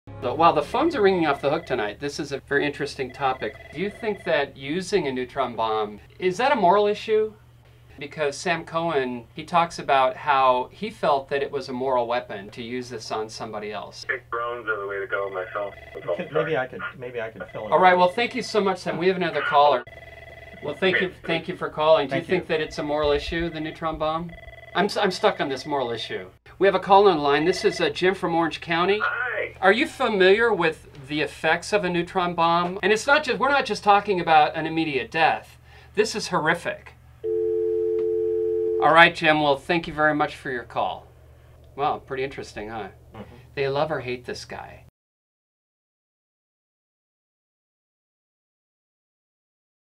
Neutron bomb- the story of a nuclear weapon designed to emit lethal radiation.
Peter Kuran – Ref. 8372